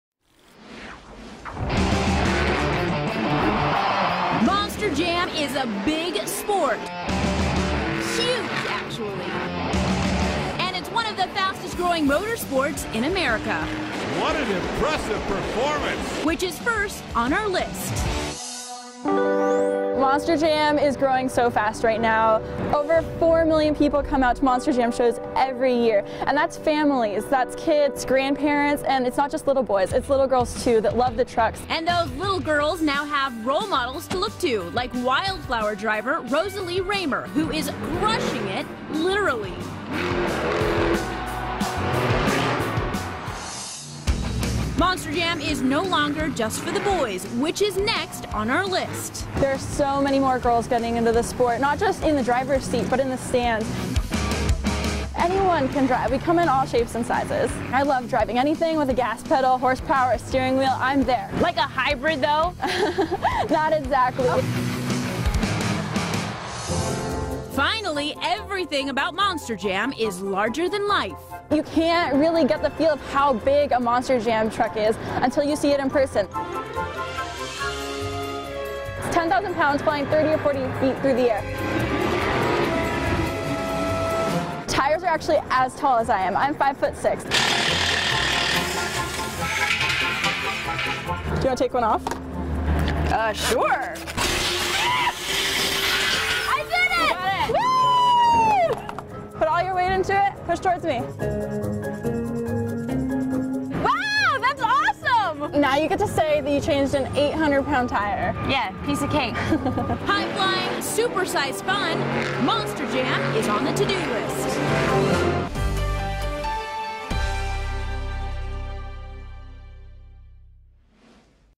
To-Do: 3 Reasons To Zoom To A Monster Jam Event
Monster Jam has been a huge sport for a long time and now it's only getting bigger and better. And it isn't just for truckers or guy's night anymore either. There's heart stopping, loud cheering, good time having fun to be had for everyone at a Monster Jam rally. So go big or go home: here are 3 reasons you need to get to a Monster Jam event.